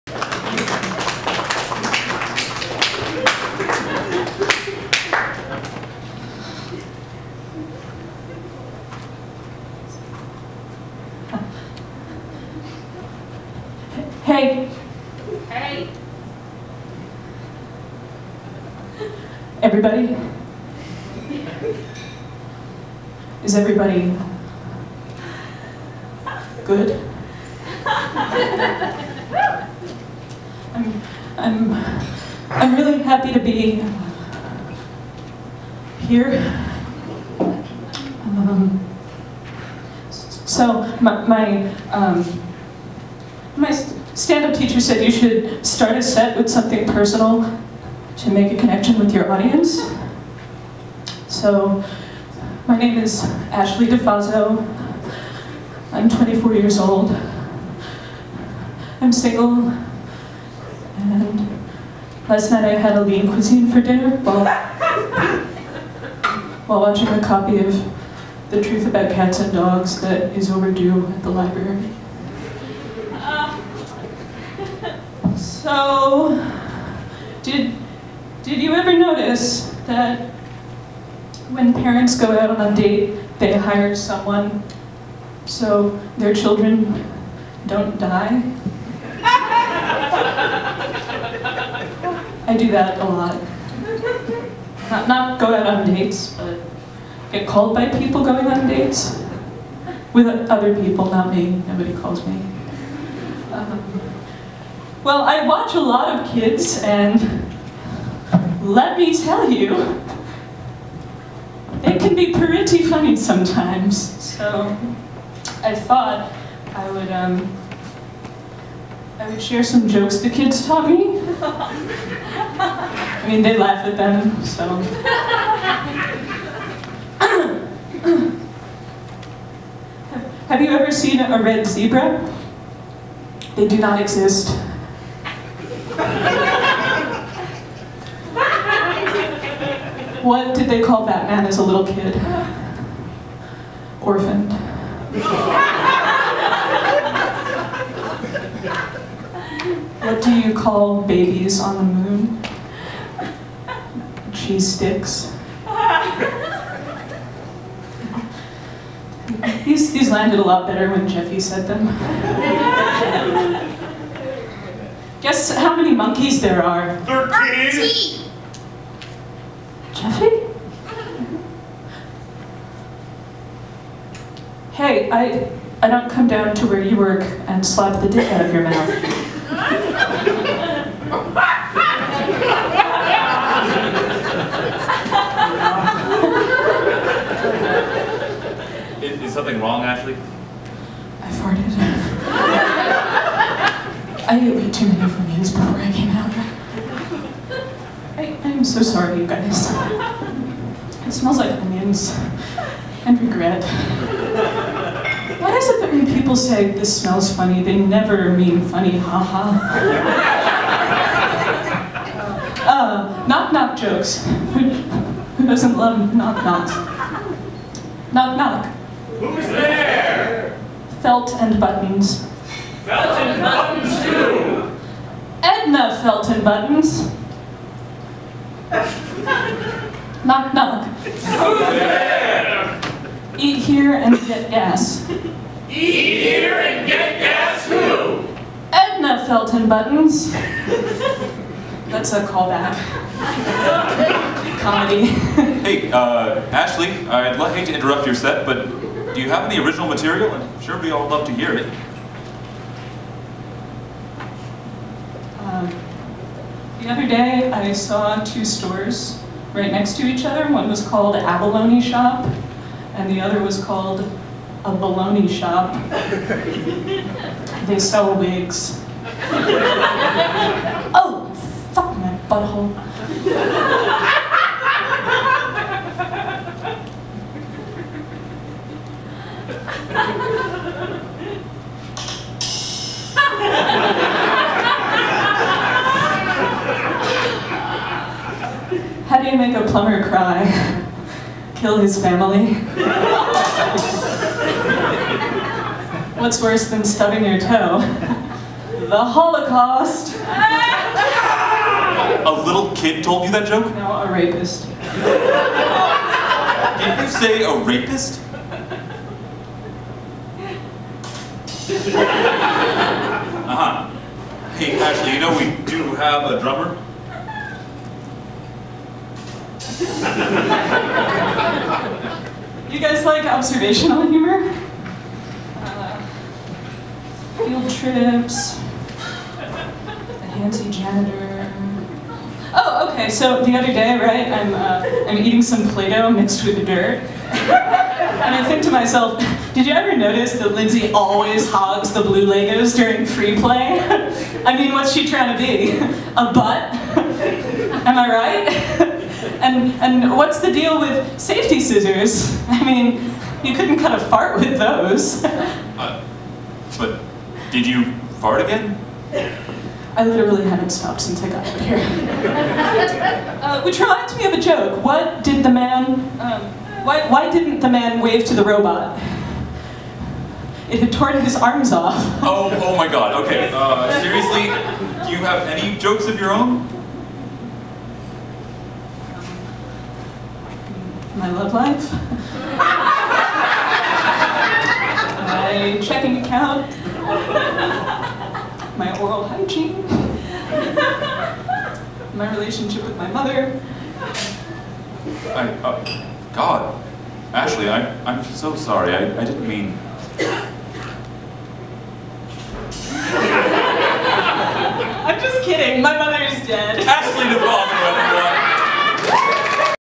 Ashleigh's Debut!
Portland comedienne, Ashleigh DeFazzo making her professional debut.  Please be gentle